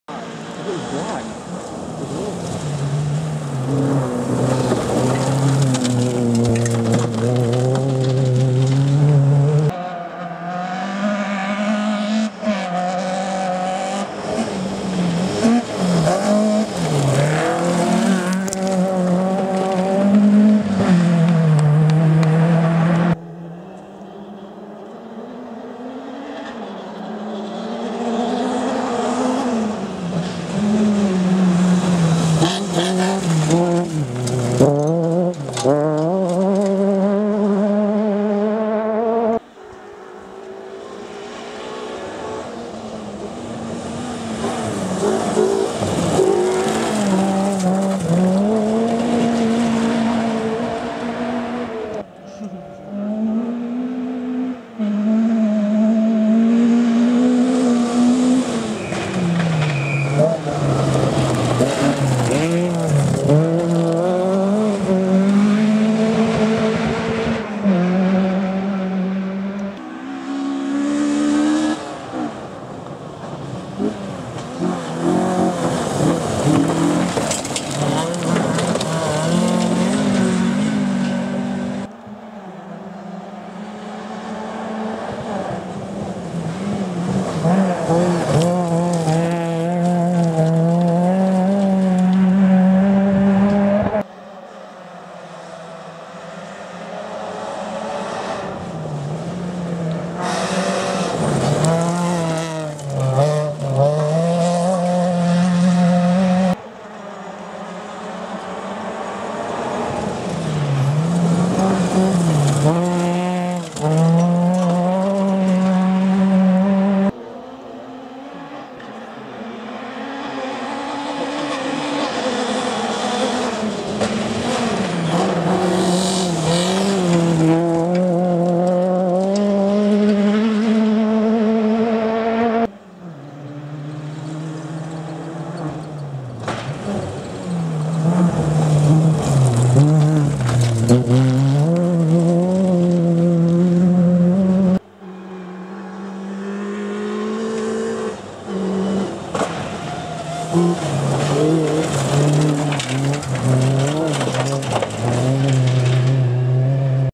Scottish Rally 2013 BRC Challenge - Shaw Hill
The BRC Challenge cars passing a tight left hand bend in the 2013 Scottish Rally - Shaw Hill Stage